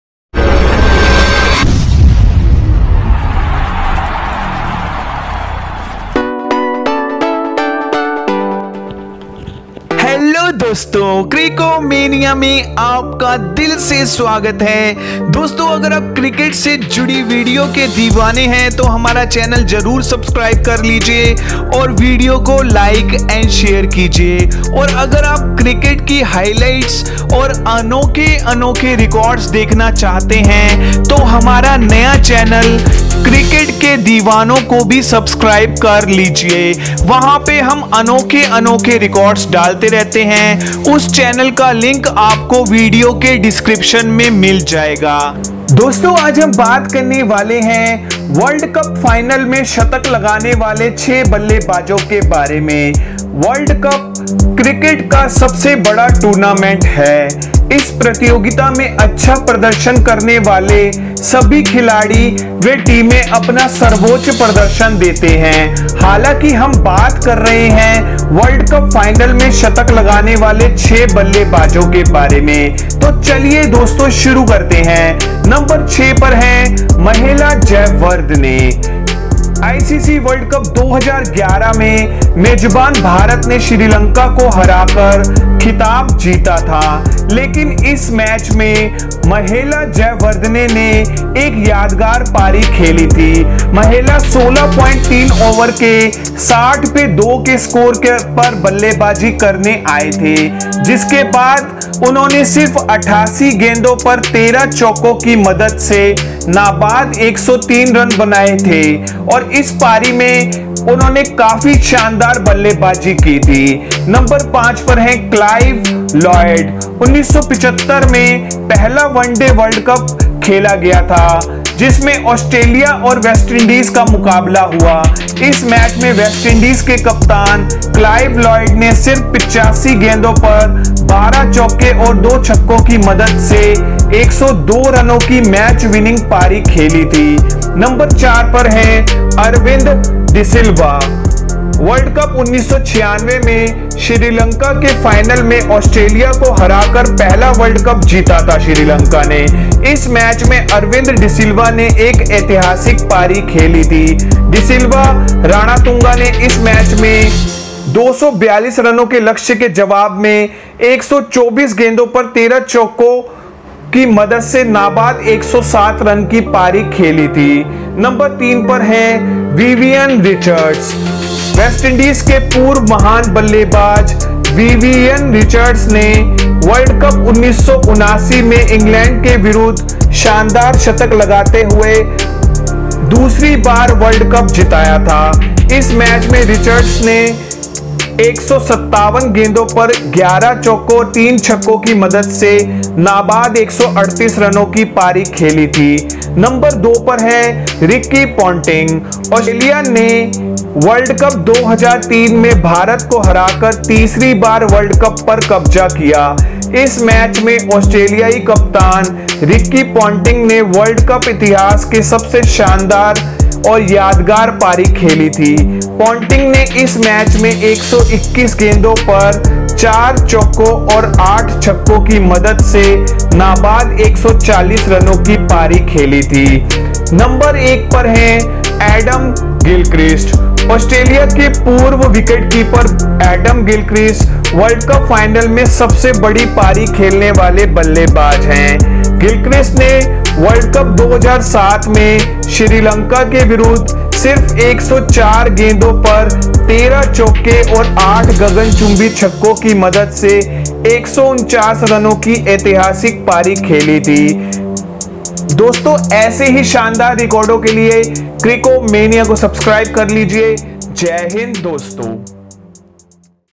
वर्ल्ड कप फाइनल मे शतक लगाने वाले | World Cup Final century by batsman| 2023 World Cup final
world cup 2023 final full highlights
world cup final century
world cup final full highlights

iss video mein bataya gaya 6 aise ballebaazo k baare mein jinhone cricket world cup k final mein shatak lagaaye hai